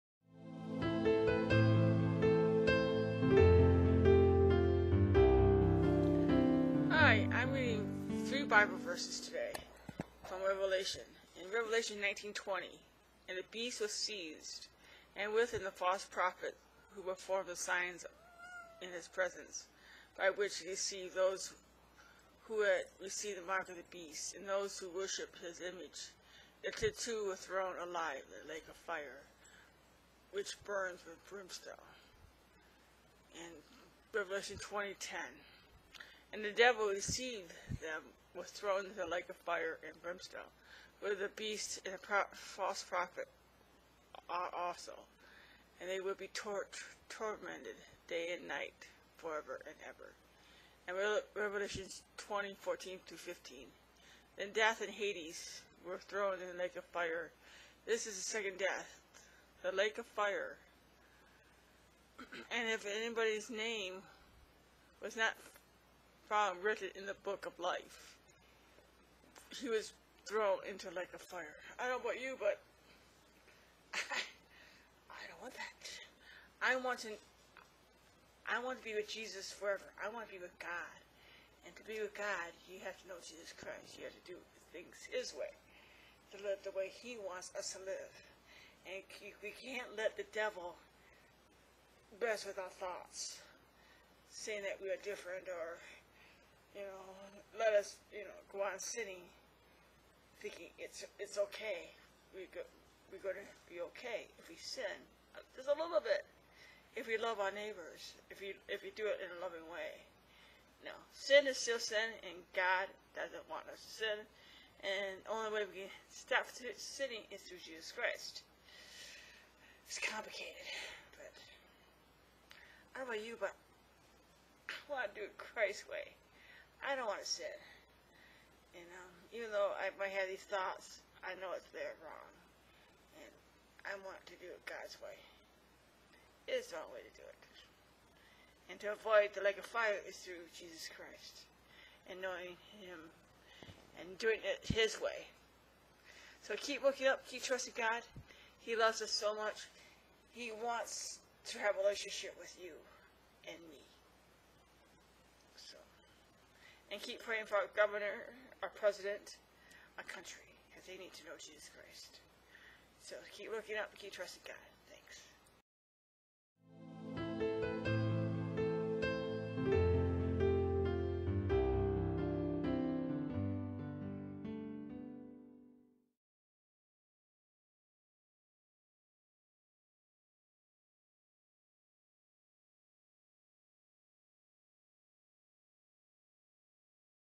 LAKE OF FIRE!
Rev_19:20  And the beast was seized, and with him the false prophet who performed the signs in his presence, by which he deceived those who had received the mark of the beast and those who worshiped his image; these two were thrown alive into the lake of fire which burns with brimstone.
Rev_20:10  And the devil who deceived them was thrown into the lake of fire and brimstone, where the beast and the false prophet are also; and they will be tormented day and night forever and ever.
Rev_20:14 -15 Then death and Hades were thrown into the lake of fire. This is the second death, the lake of fire.  And if anyone's name was not found written in the book of life, he was thrown into the lake of fire.